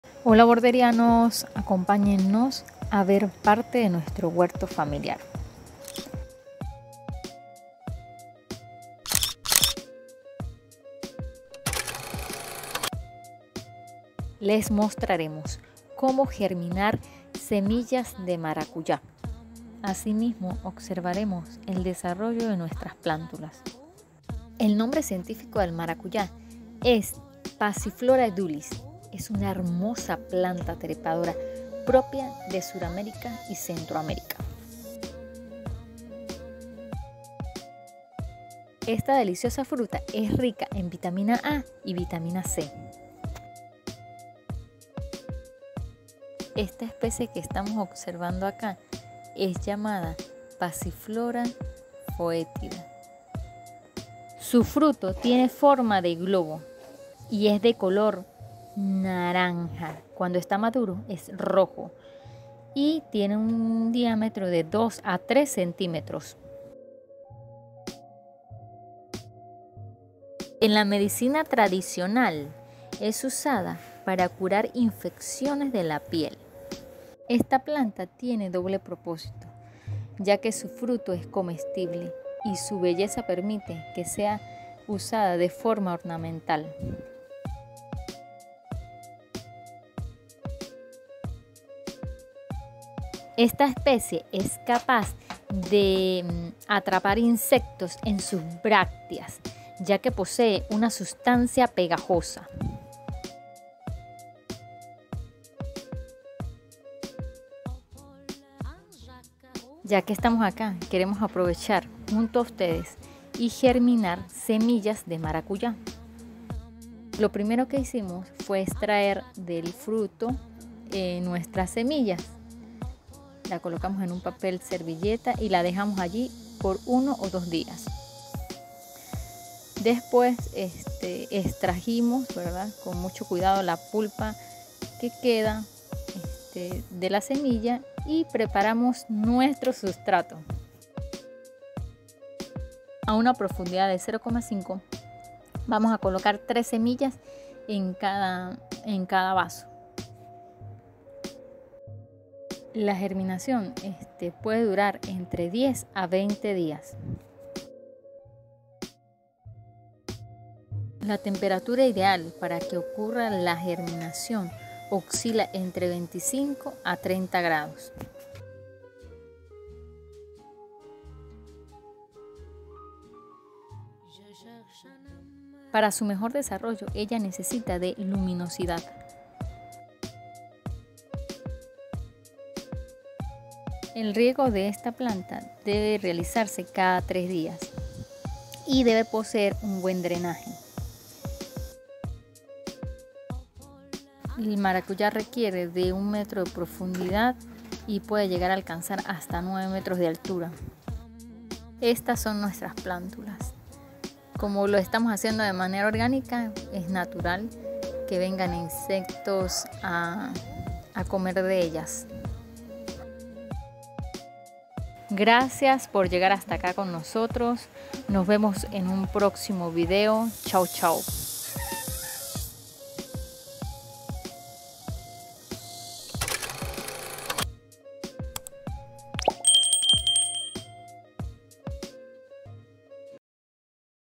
MARACUYÁ(PASSIFLORA EDULIS) VS MARACUYÁ SILVESTRE (PASSIFLORA FOETIDA)
MARACUYÁ.  Este delicioso  fruto posee distintas variedades, acá mostramos dos que poseemos en nuestro huerto. Igualmente, mostramos nuestra experiencia germinando  una de ellas. Espero les sea útil nuestra  experiencia, lo hacemos con cariño y respeto para ustedes.

Un abrazo y que la naturaleza los bendiga.